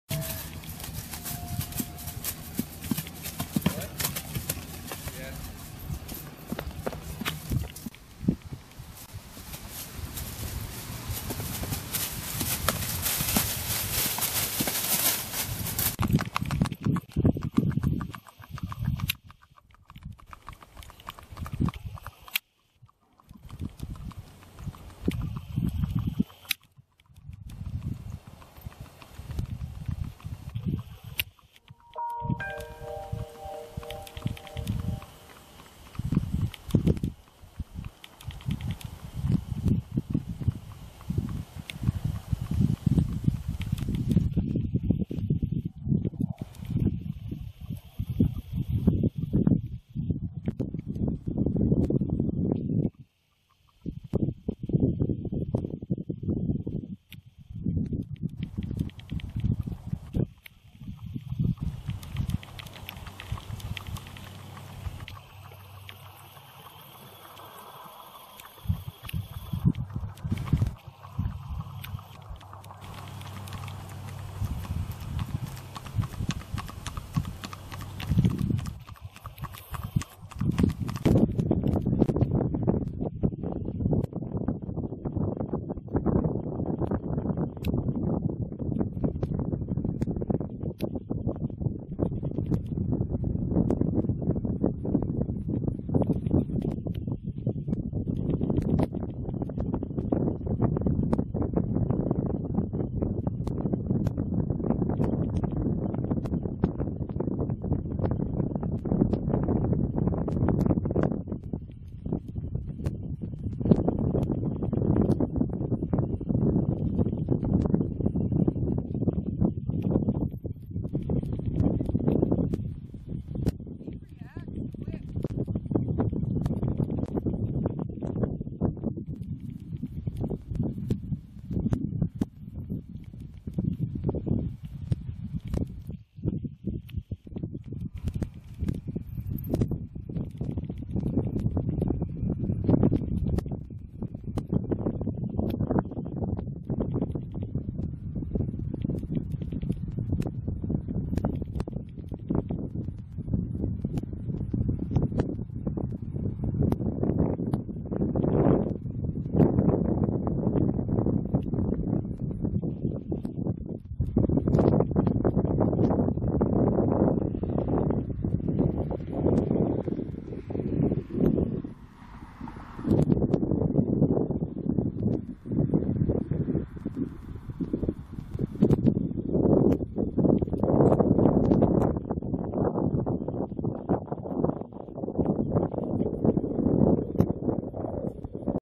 Jennifer and Amigo riding at Summersville Lake
Jennifer's first time ever riding Amigo.  She loved his smooth gait.